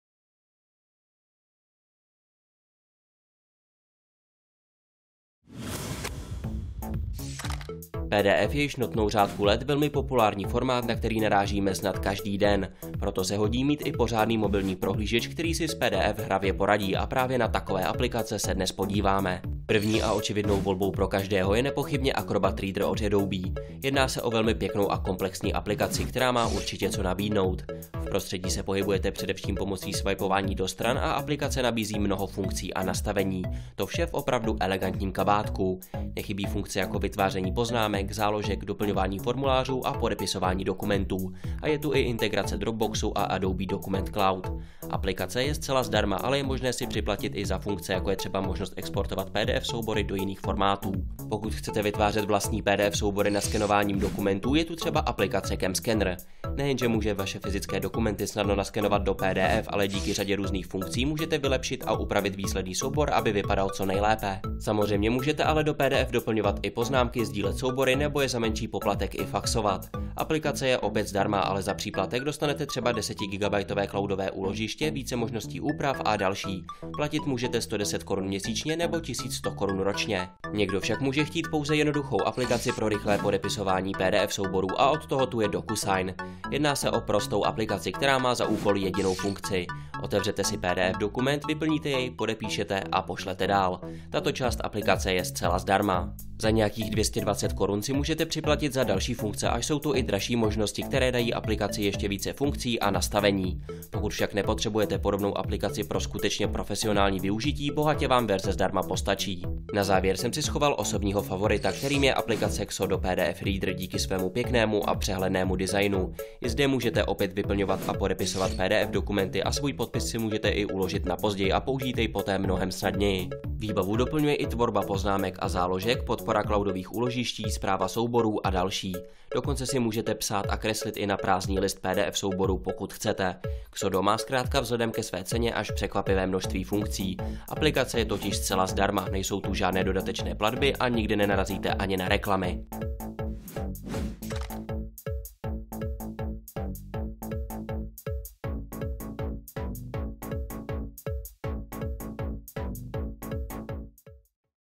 Nejlepší aplikace pro práci s PDF (Top Apps)
- Dnes si představíme čtyři aplikace, které vám usnadní práci s formátem PDF
- Ukážeme si základní prohlížeče, editory, skener a podepíšeme si i pár dokumentů
- Všechny aplikace jsou přitom v základní verzi zcela zdarma